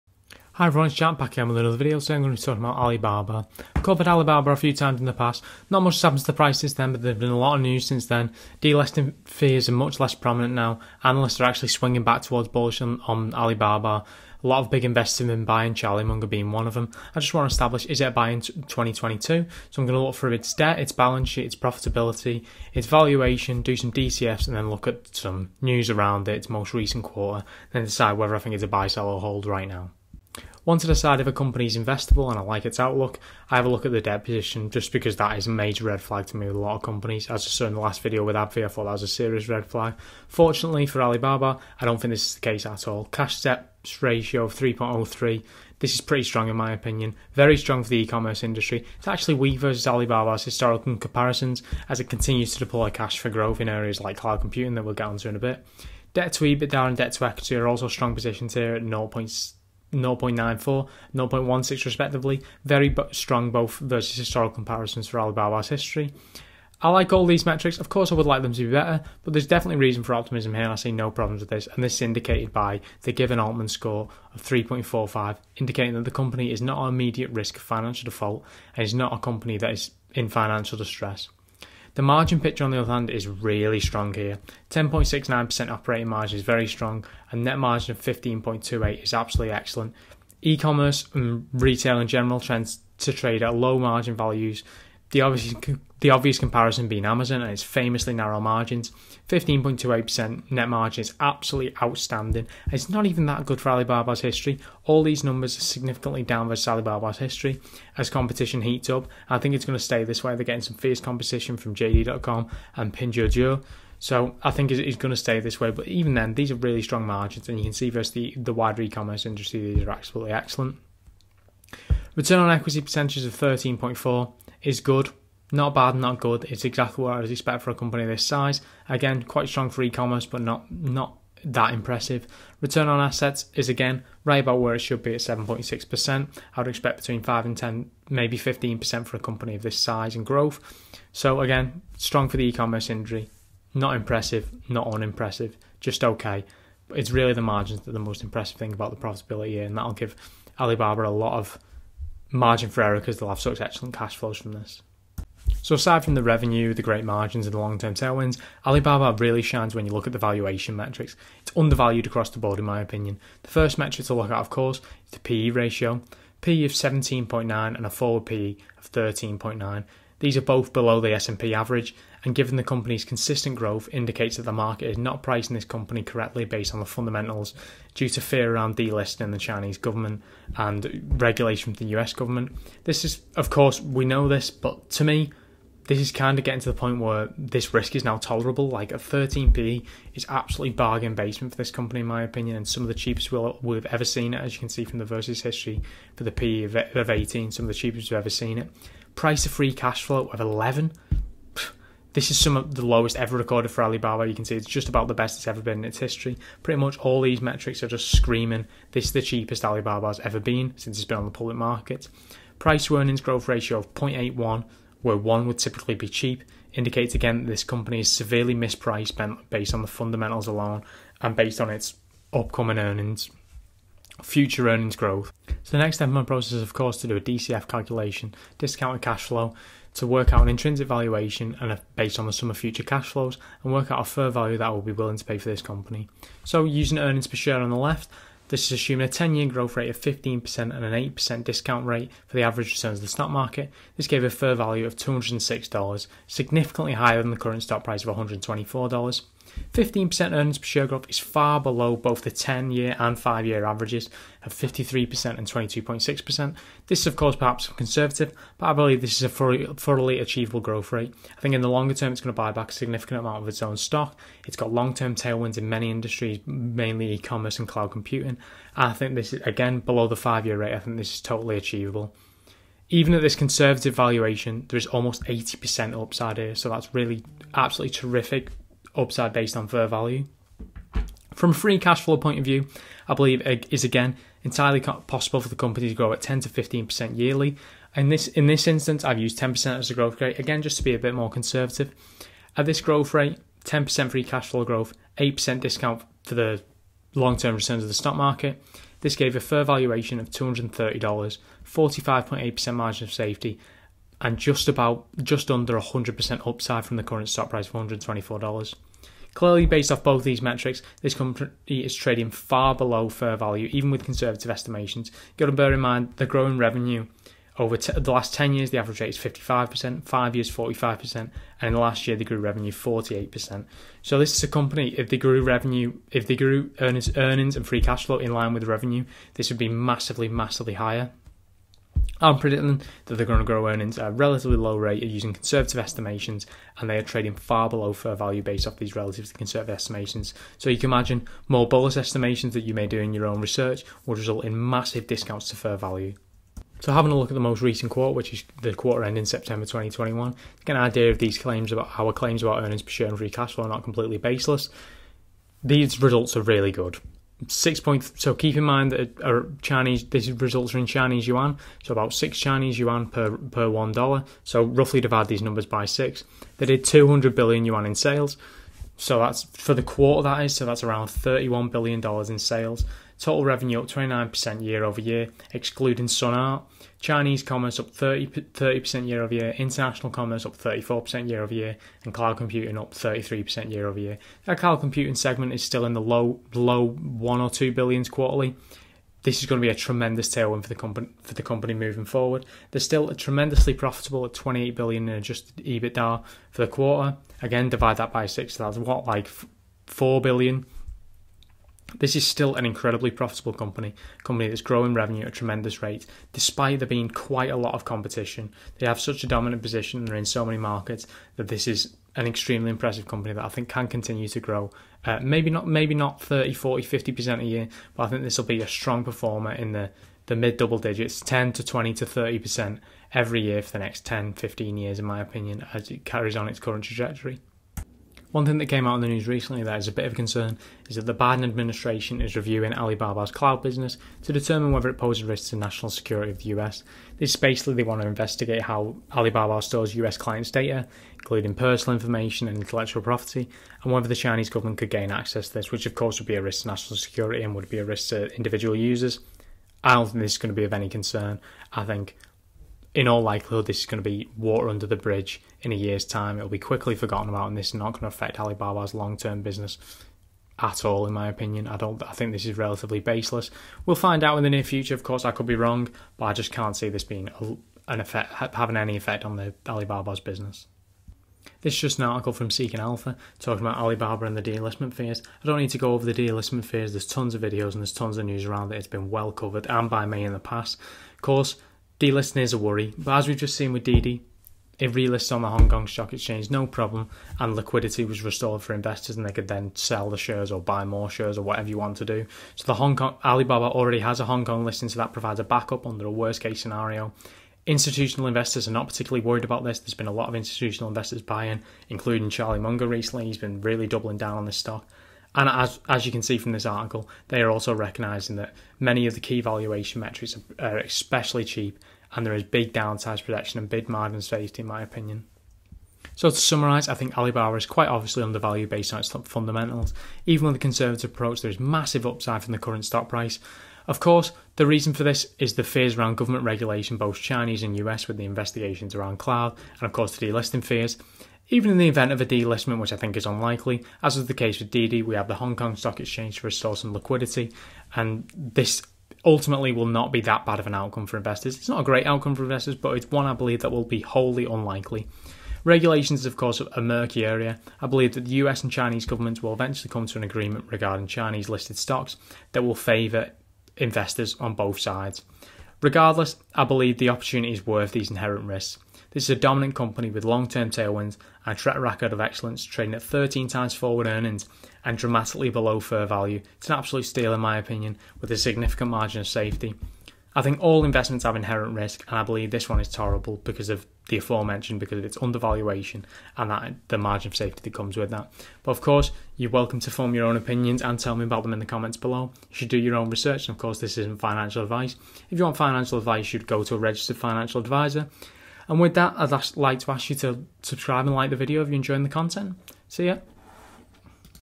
Is Alibaba Stock a Buy in 2022? | 100% Upside? | BABA Stock Analysis
Alibaba stock is down over 50% from yearly highs, despite excellent results. Delisting, regulation, hysteria all contributing. Is this all overblown or is the fear warranted. Is Alibaba a buy, sell or hold for 2022?